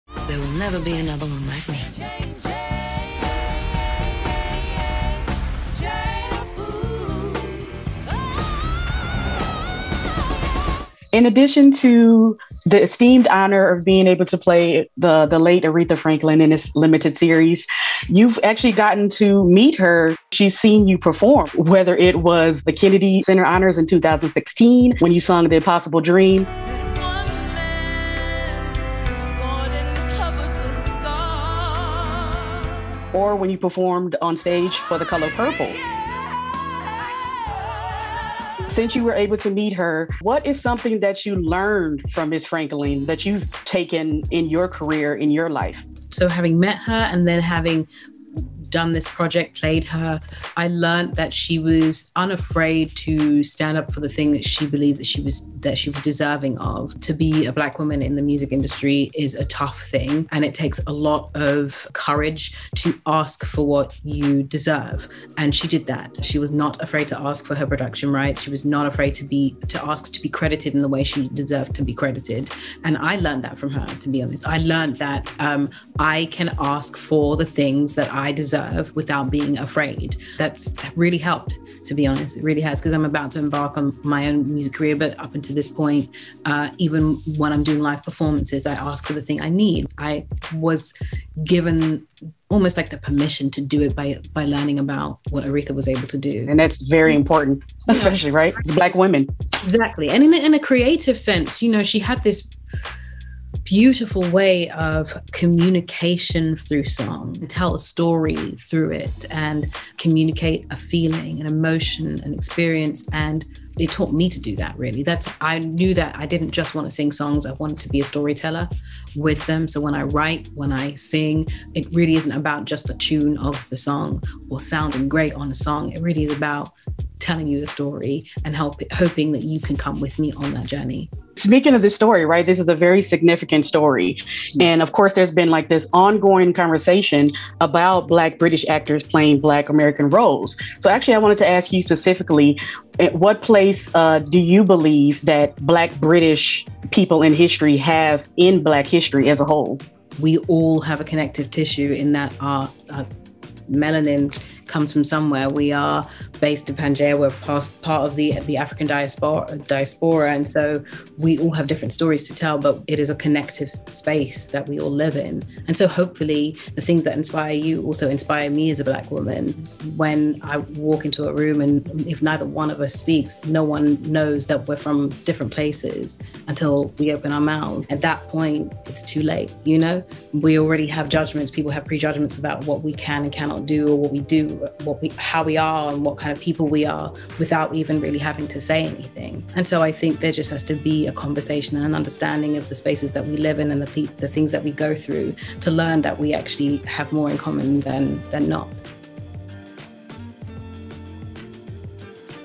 What Did Cynthia Erivo Learn From Aretha Franklin? To Ask for What She Deserves Without Being Afraid
This upcoming weekend, Cynthia Erivo will be debuting her portrayal of the late Aretha Franklin in the upcoming limited series from National Geographic, Genius: Aretha.

Erivo has previously spoken about the fact Franklin saw her perform before—at an onstage performance of The Color Purple as well as at the 2016 Kennedy Center Honors—so when I recently sat down with her to discuss the esteemed honor of portraying the late legend, I wanted to know what she learned from her. It was an especially timely conversation to have during Women’s History Month and as Erivo embarks on her own music career.